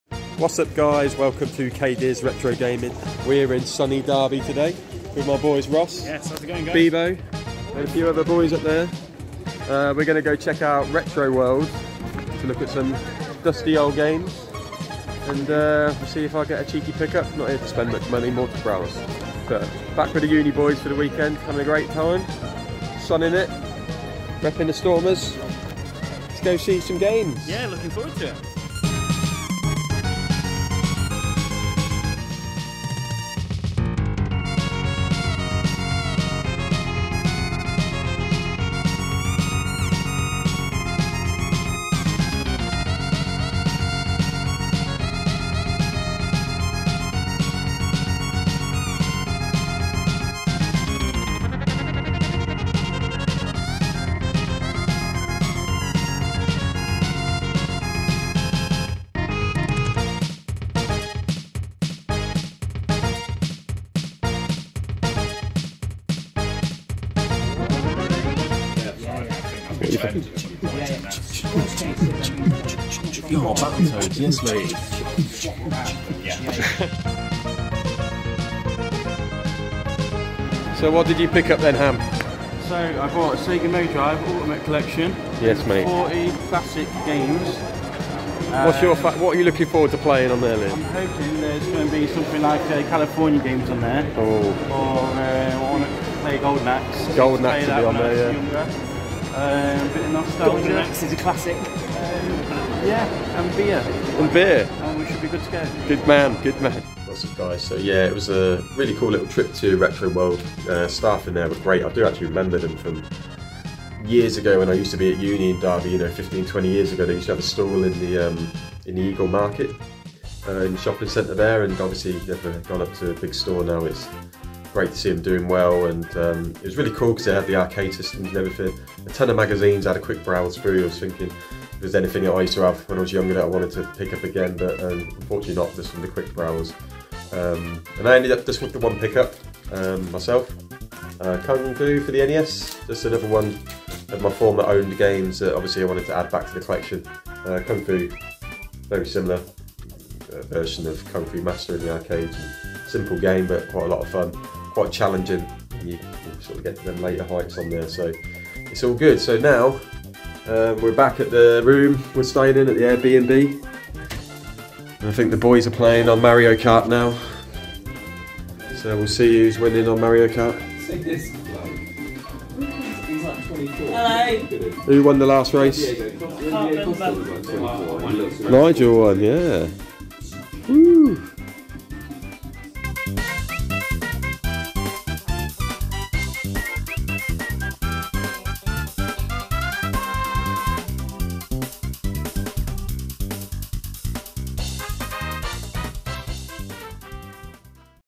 Trip to Derby's Retro World
Took a quick trip to Derby's Retro World for a browse with my old Uni pals during our reunion weekend.